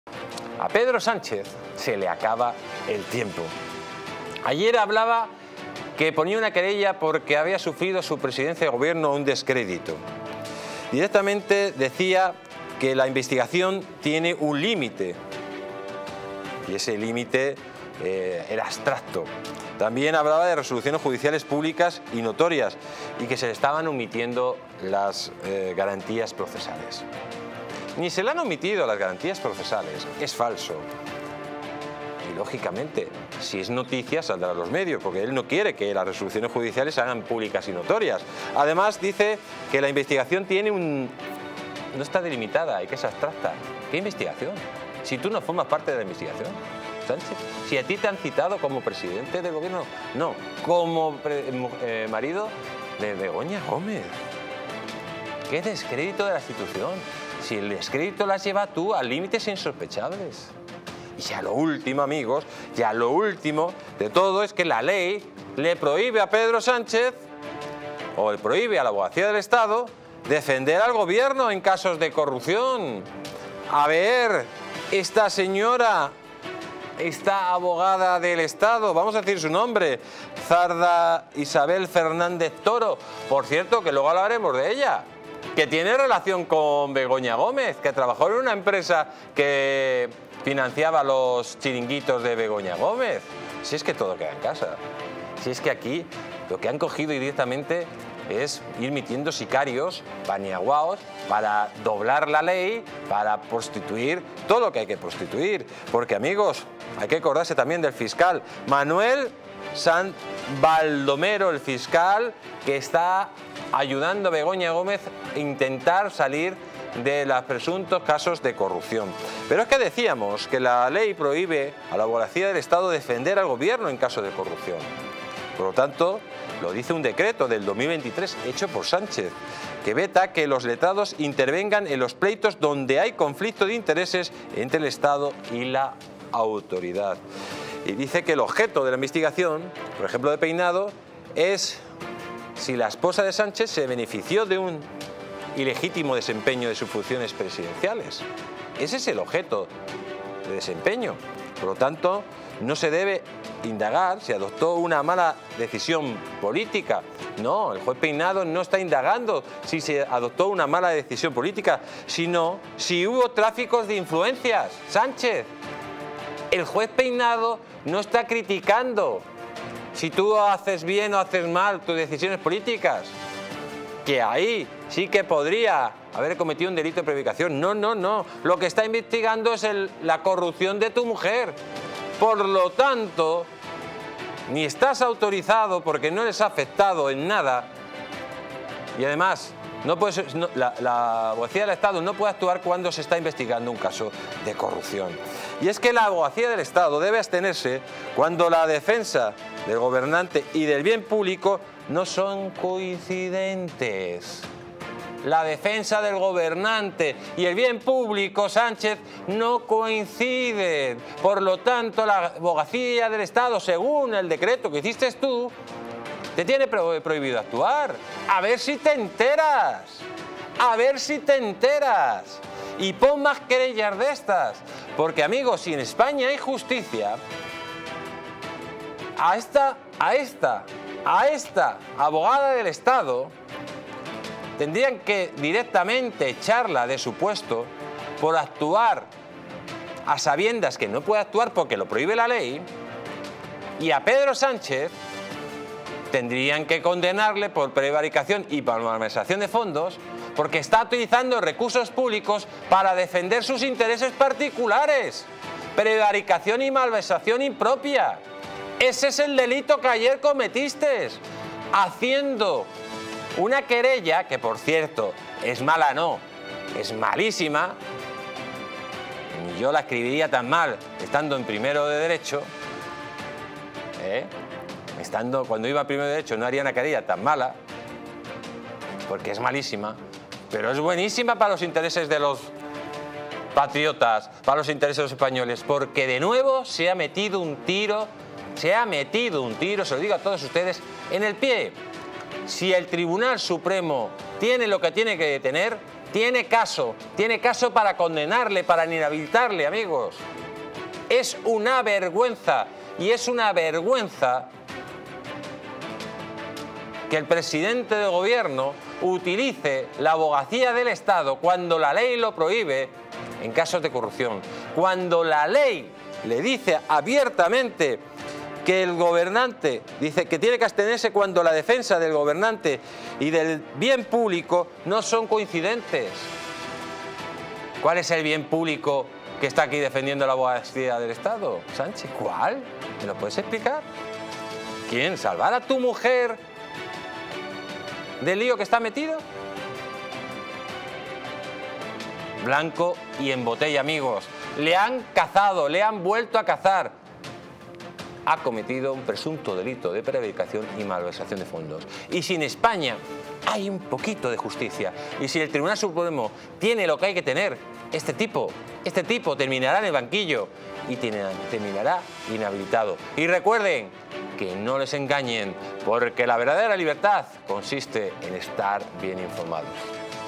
🔴EDITORIAL del DÍA🔴LA LEY PROHIBE A LA ABOGACÍA DEL ESTADO DEFENDER A SÁNCHEZ EN CASOS DE CORRUPCIÓN
En este editorial del día, discutimos la ley que prohíbe a la abogacía del estado defender a Sánchez en casos de corrupción. ¡No te pierdas este importante debate en DistritoTV!